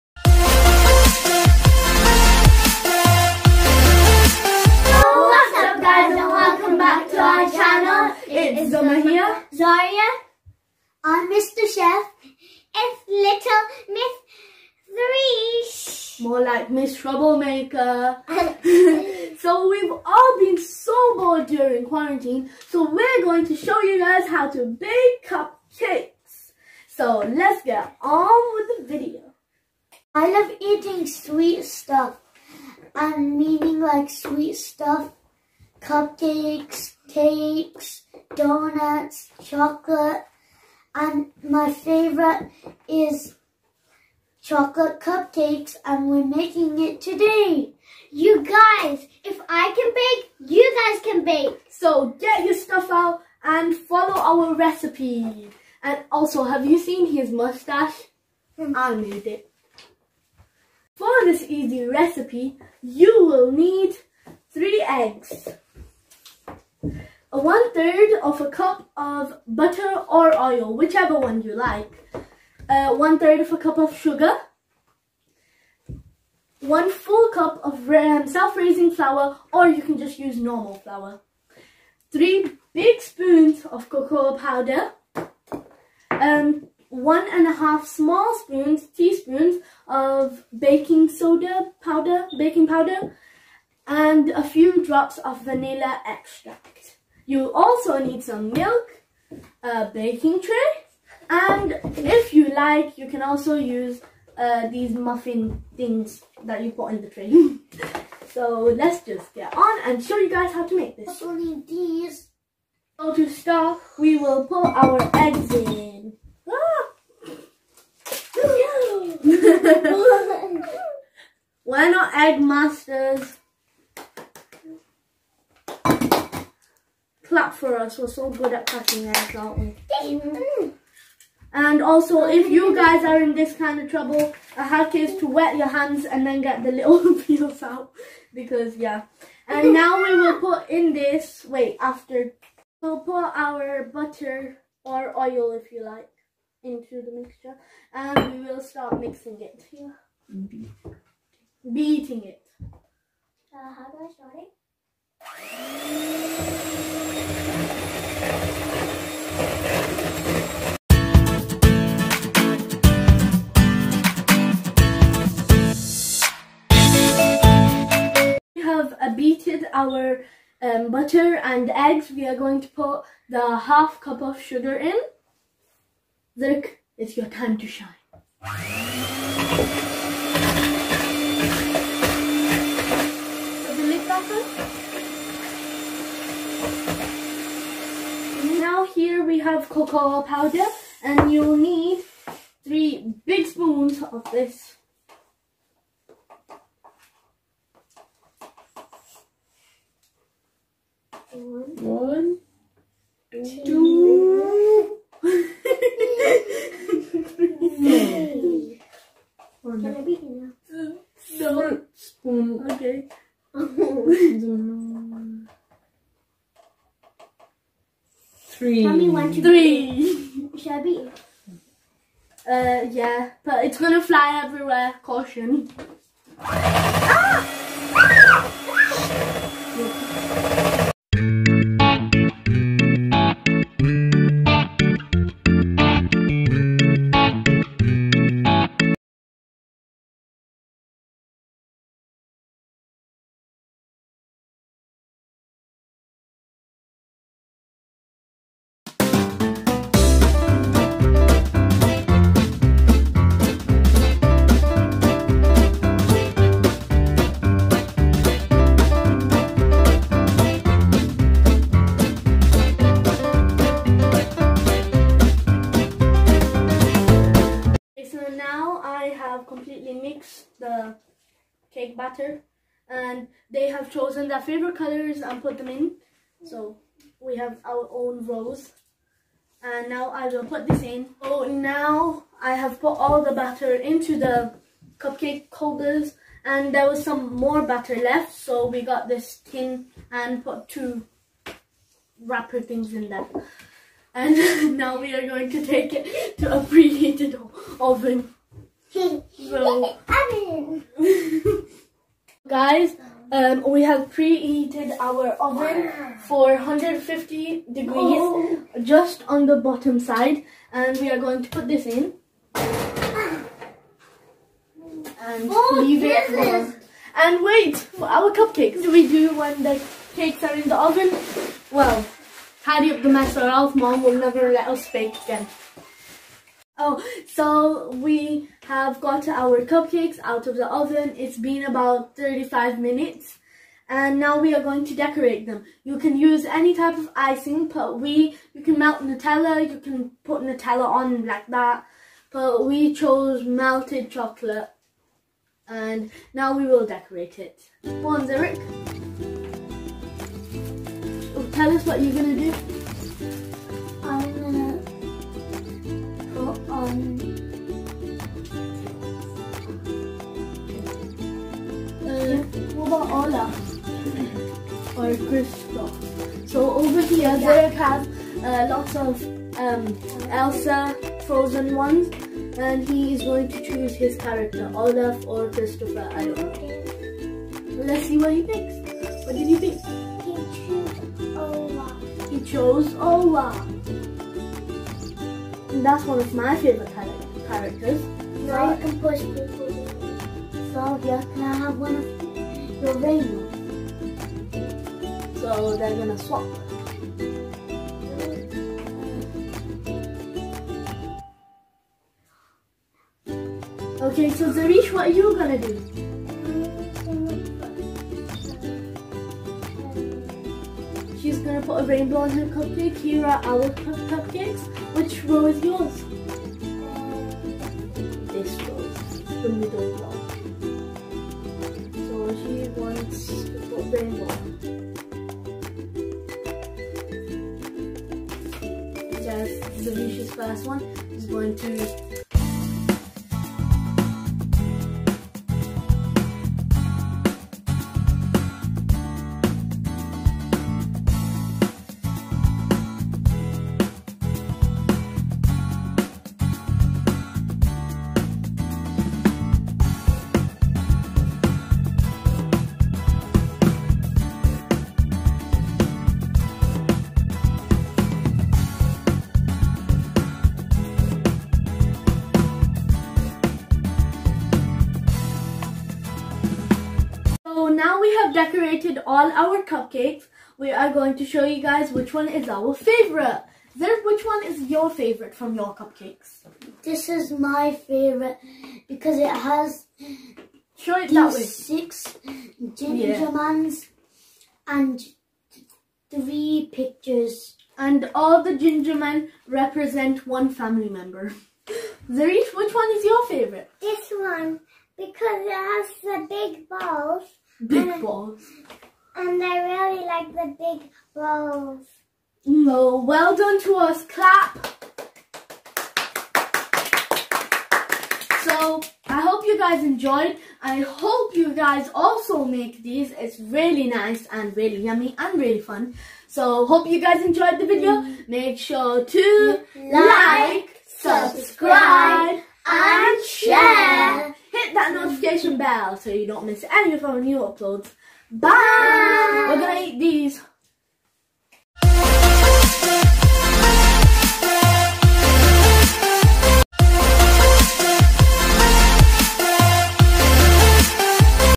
Zzzz Bake Cupcakes|The Life Of Zzzz
Today we made Cupcakes! Now I don't want to see you sitting around. Get up and get all your  ingredients out, and start baking!

Ingredients:

•Eggs      3
•Butter      1/3
•Sugar      1/3
•Flour      1 cup
•Cocoa Powder      3 table spoons
•Baking Powder      1+1/2 tea spoons
•Vanilla Extract      Few Drops

Now sit and enjoy!